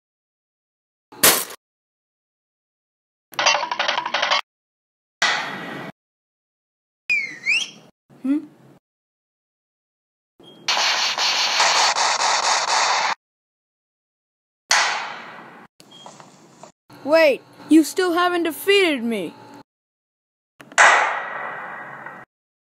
John wick part 1
I will be making John wick part two when I get more mini figs so wait about let’s see umm 5 months or so 😐